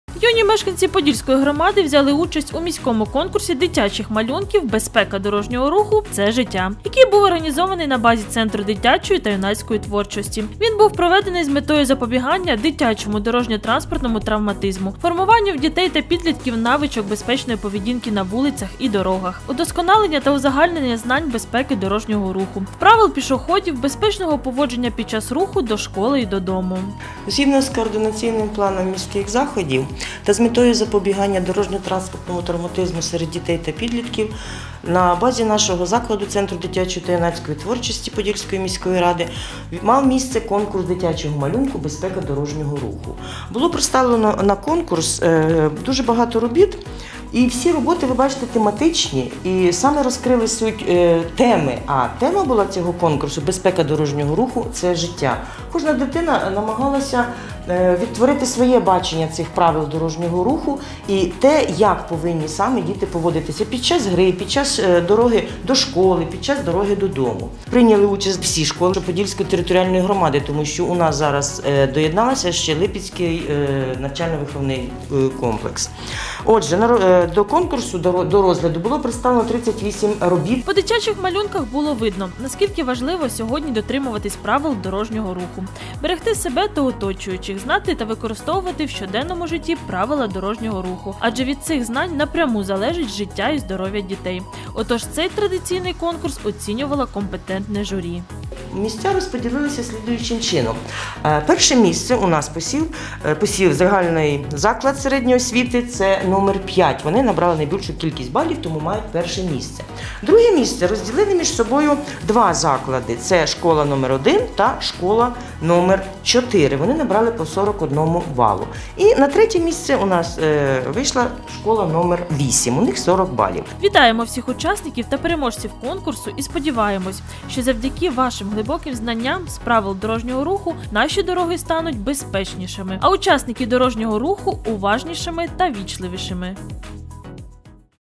Конкурс дитячих малюнків «Безпека дорожнього руху - це життя!»
У Центрі дитячої та юнацької творчості був проведений конкурс дитячих малюнків «Безпека дорожнього руху-це життя!». Подробиці дивіться у сюжеті.